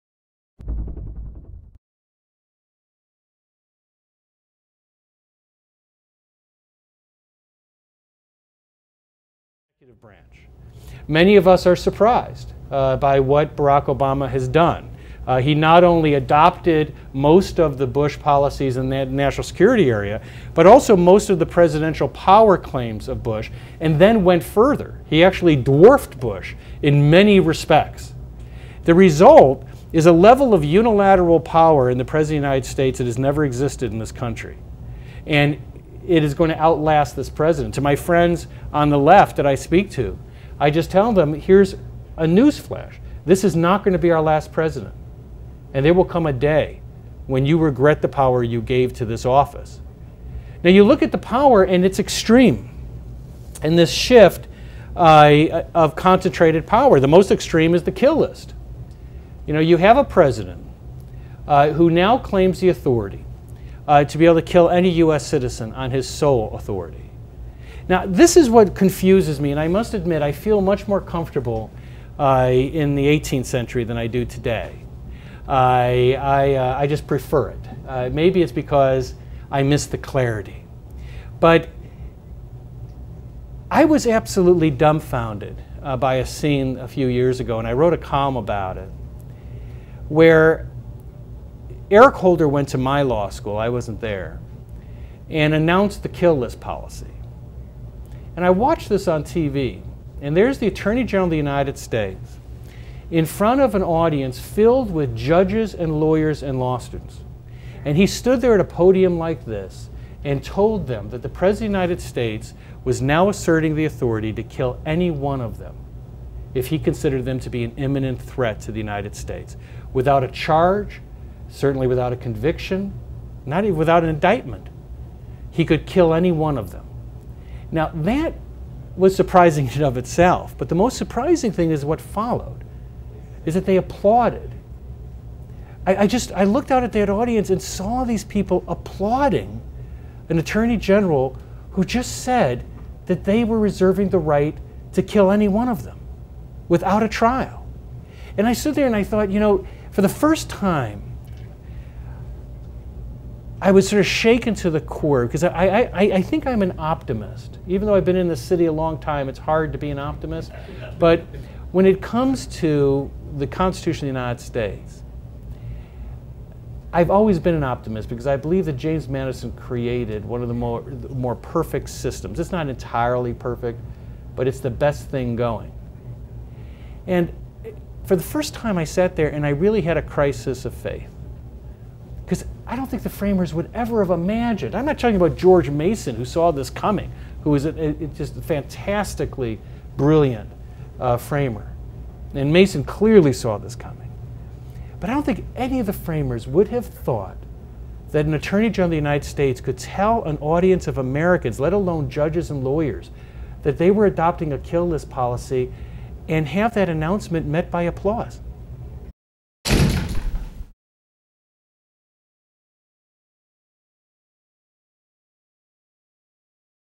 Jonathan Turley - Wildly Expanded Executive Powers
Law professor Jonathan Turley explains how the Obama Administration wildly expanded executive powers to the point where the American president can now order any citizen he wishes to be killed.